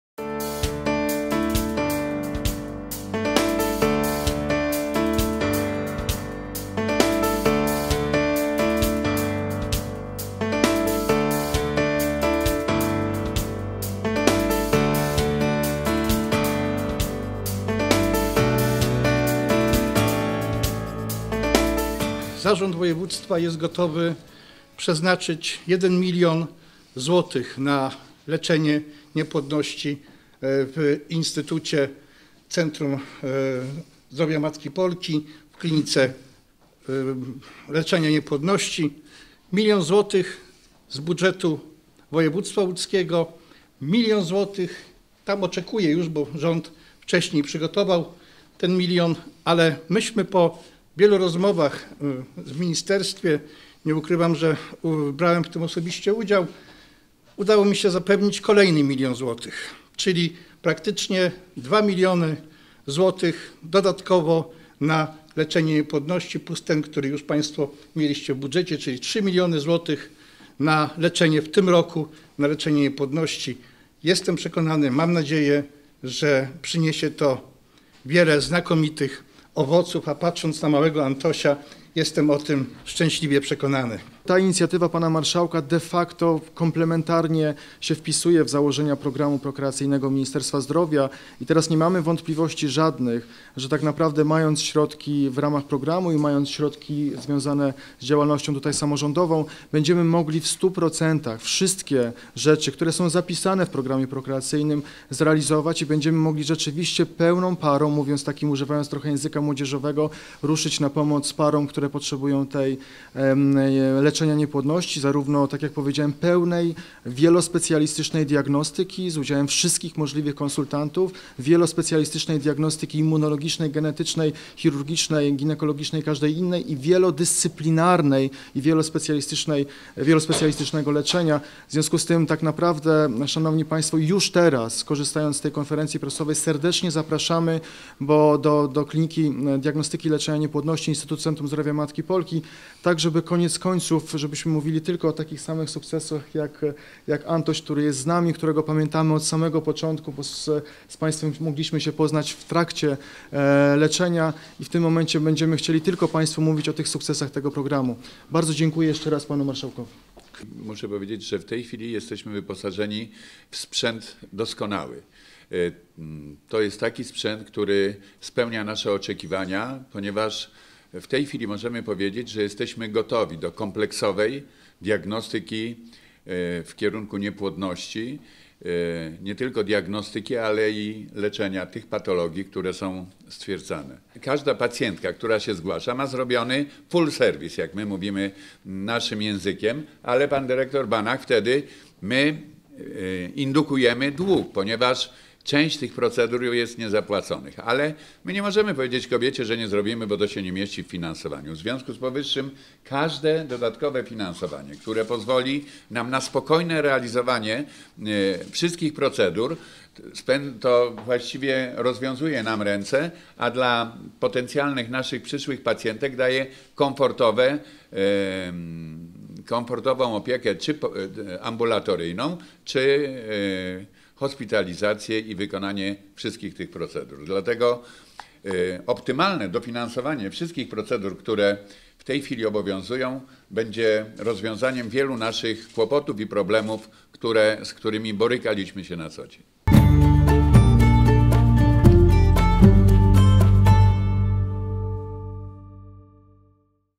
💰 Miliony złotych na leczenie niepłodności!
Milion złotych trafi z Urzędu Marszałkowskiego Województwa Łódzkiego do Instytutu Centrum Zdrowia Matki Polki w Łodzi na leczenie niepłodności. Dodatkowo dzięki staraniom Marszałka WŁ i ścisłej współpracy z rządem, Ministerstwo Zdrowia przekaże na ten cel 2 mln złotych.

Podczas konferencji prasowej szczegóły przedstawili: marszałek województwa Grzegorz Schreiber oraz dyrektor ICZMP prof. Maciej Banach i kierownik Kliniki Ginekologii, Rozrodczości i Terapii Płodu oraz Diagnostyki Leczenia Niepłodności prof. Krzysztof Szaflik.

Program leczenia niepłodności realizowany w Klinice Diagnostyki i Leczenia Niepłodności w Instytucie Centrum Zdrowia Matki Polki opiera się na ścisłej współpracy i kompleksowym działaniu lekarzy różnych specjalizacji związanych z medycyną rozrodu: andrologami, genetykami ,endokrynologami, immunologami. Lekarze obejmują leczeniem pary, bowiem przyczyna niepłodności musi być poszukiwana u obojga. Świadczenia realizowane w ramach programu to wyłącznie metody o skuteczności popartej badaniami naukowymi. Początek leczenia to głęboka i wielowymiarowa pogłębiona diagnostyka . Pozwala ona na wykrycie schorzeń negatywnie wpływających na płodność: u kobiet to np. zespół policystycznych jajników, endometrioza, torbiele, a u mężczyzn np. zaburzenia wpływające na liczbę plemników w nasieniu. Dzięki współpracy lekarzy szybciej i trafniej można postawić diagnozę. Pacjent jest traktowany wielowymiarowo, a wyniki badań są konsultowane i analizowane jednocześnie z różnych perspektyw.

Wiele problemów zdrowotnych utrudniających zajście w ciążę można skutecznie leczyć ambulatoryjnie  poprzez zabiegi operacyjne, farmakologię oraz pomoc psychologiczną. Warto nadmienić, że  według naukowych doniesień 85 do 90% par, które dotyka problem niepłodności może go rozwiązać właśnie dzięki kompleksowemu programowi leczenia bezpłodności.

Do grudnia 2018 roku Klinika objęła opieką 402 pary, 37 mogło pochwalić się ciążą. Niezwykle istotne jest też to, że opieka nad parą nie kończy się w momencie zajścia w ciążę. Rodzice pozostają pod ścisłą opieką specjalistów tak, by zminimalizować ryzyko utraty ciąży.